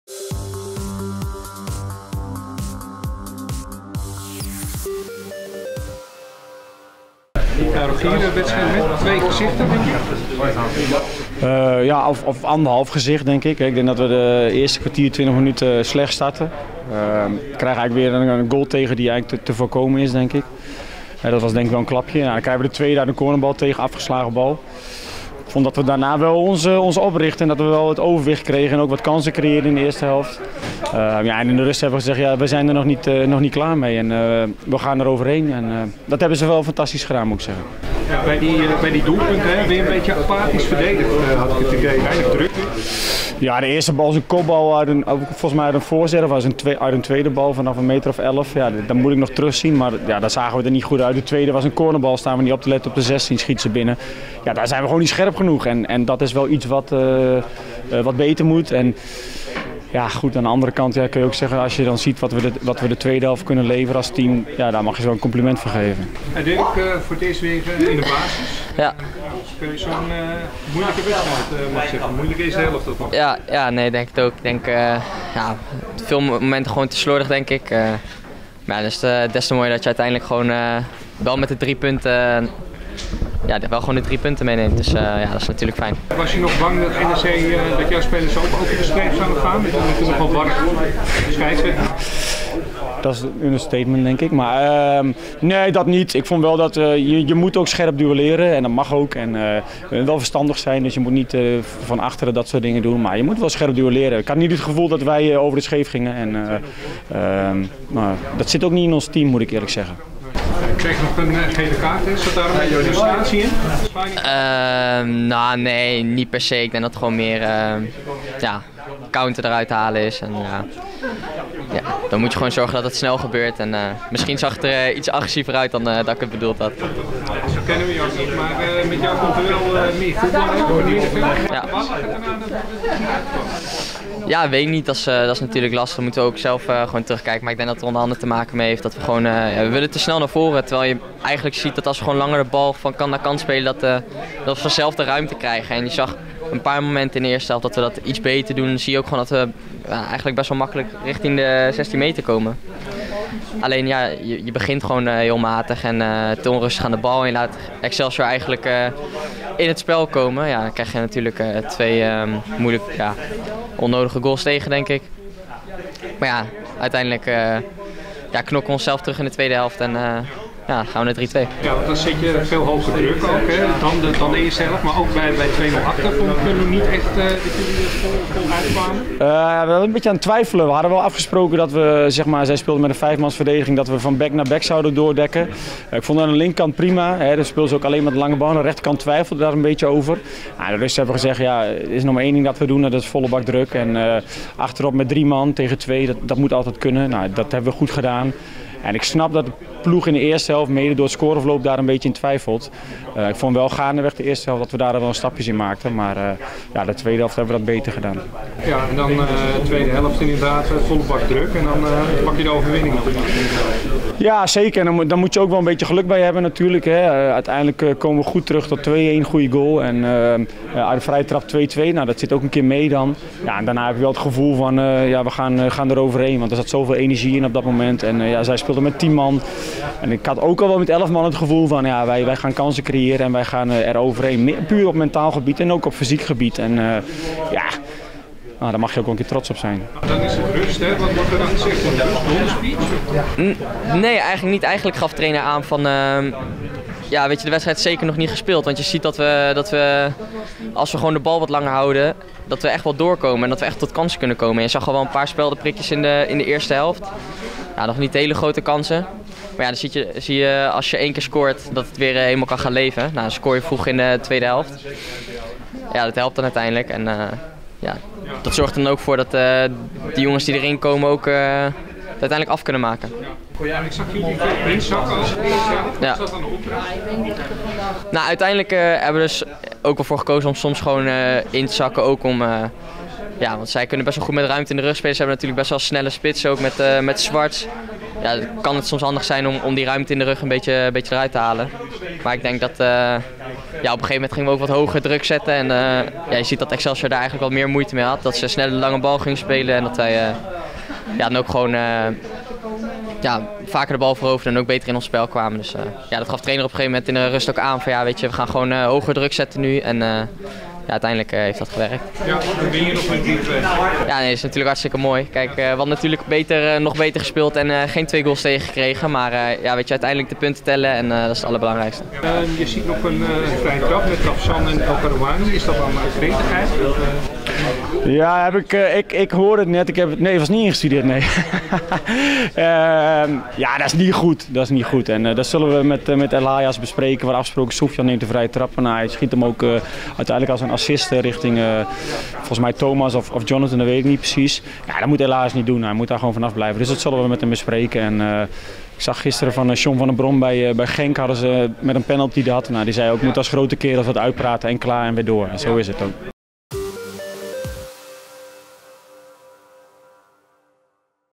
Nabeschouwing: NEC toont veerkracht tegen Excelsior
Na een 0-2 achterstand won NEC vrijdagavond alsnog,